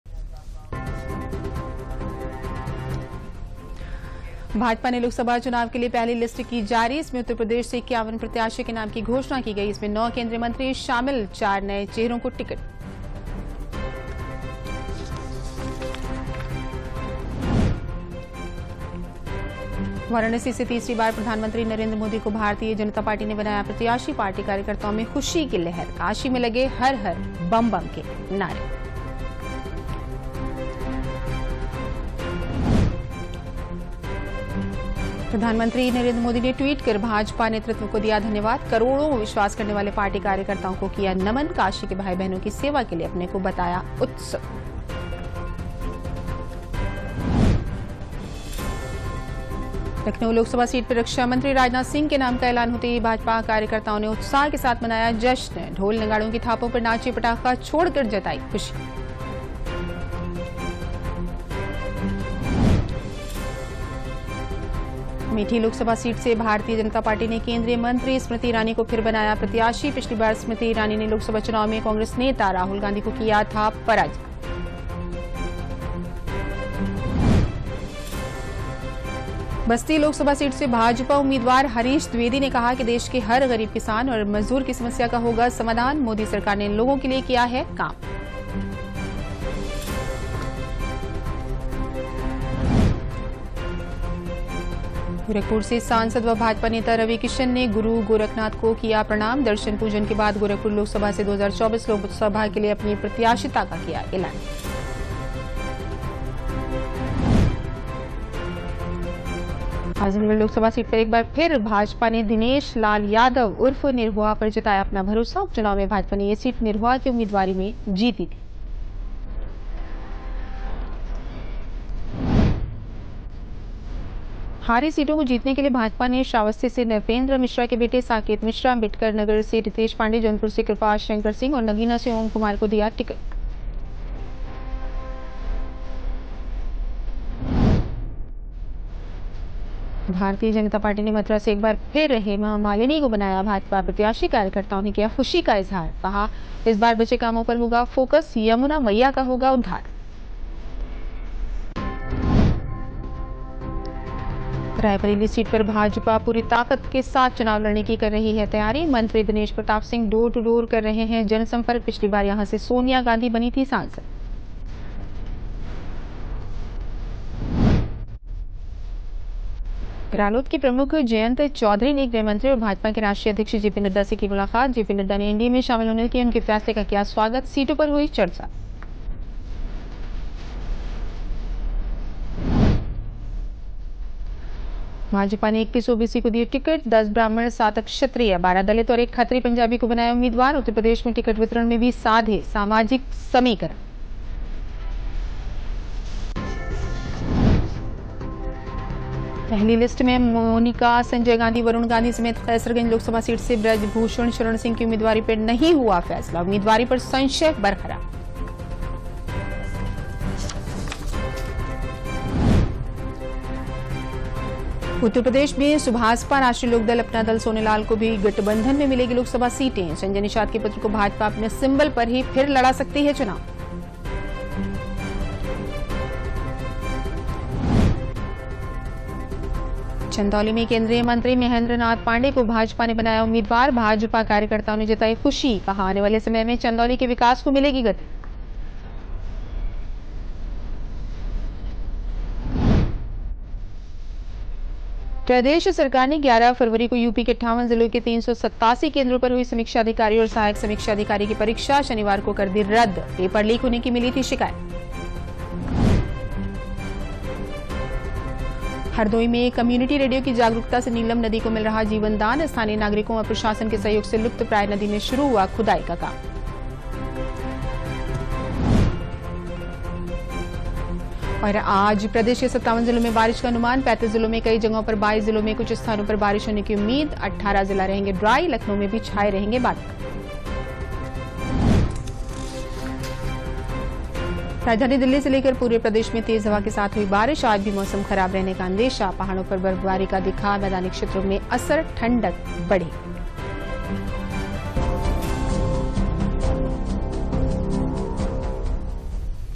देखिए देश और प्रदेश की 20 बड़ी ख़बरें
1- भाजपा ने लोकसभा चुनाव के लिये जारी की पहली सूची
2-वाराणसी से तीसरी बार पीएम मोदी को भाजपा ने बनाया प्रत्याशी
3-प्रधानमंत्री नरेंद्र मोदी ने ट्वीट कर भाजपा नेतृत्व को दिया धन्यवाद
4-भाजपा कार्यकर्ताओं ने उत्साह के साथ मनाया जश्न
5-भाजपा ने केन्द्रीय मंत्री स्मृति ईरानी को फिर बनाया प्रत्याशी
6-देश के हर गरीब किसान की समस्या का होगा समाधान- हरीश दिवेदी
7- सांसद व भाजपा नेता रवि किशन ने गुरु गोरखनाथ को किया प्रणाम
8-  भाजपा ने दिनेश लाल यादव उर्फ निरहुआ पर जताया अपना भरोसा
9- हारी सीटों को जीतने पर भाजपा का लक्ष्य
10- भाजपा ने मथुरा से एक बार फिर हेमा मालिनी को बनाया भाजपा प्रत्याशी
11-रायबरेली सीट पर भाजपा पूरी ताकत के साथ चुनाव लड़ने की कर रही है तैयारी
12- जयंत चौधरी ने गृहमंत्री और जेपी नड्डा से की मुलाकात
13- भाजपा ने 21 ओबीसी को दिए टिकट
14- मेनिका, वरूण और बृजभूषण पर अभी नहीं हुआ फैसला
15- संजय निषाद के पुत्र को भाजपा अपने सिबंल पर ही फिर लड़ा सकती है चुनाव
16- चंदौली में  केन्द्रीय मंत्री महेन्द्रनाथ पांडेय को भाजपा ने बनाया उम्मीदवार
17- समीक्षा अधिकारी/सहायक समीक्षा अधिकारी प्रारंभिक परीक्षा निरस्त
18- हरदोई में  कम्युनिटी रेडियो की जागरूकता से नदी को मिल रहा जीवनदान
19- आज प्रदेश के 57 जिलों में बारिश का अनुमान
20-  पूरे प्रदेश में तेज हवाओं के साथ हुई बारिश